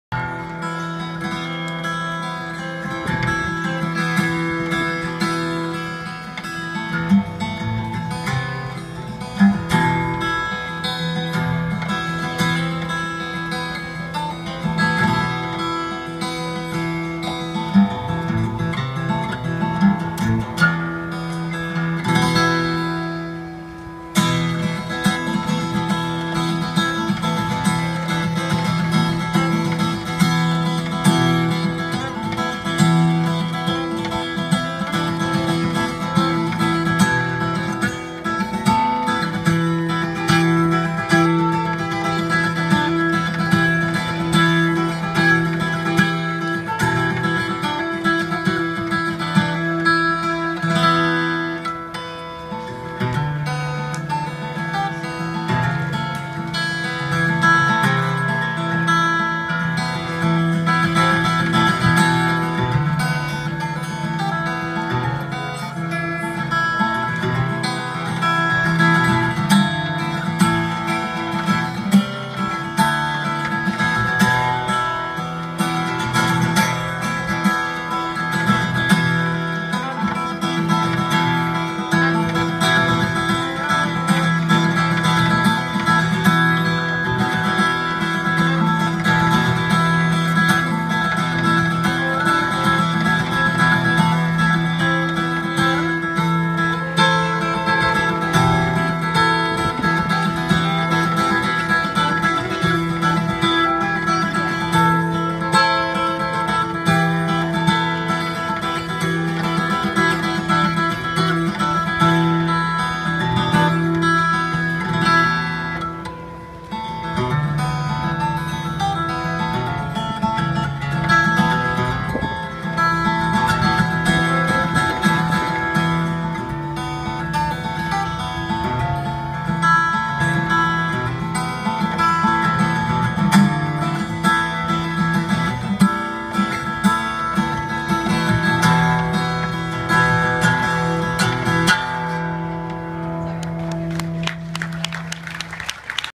Berlin Fiddler's Convention 2015
Guitar competition.  I did a spontaneous thing and entered this at the last minute even though I just had my travel guitar.  Original DADGAD song.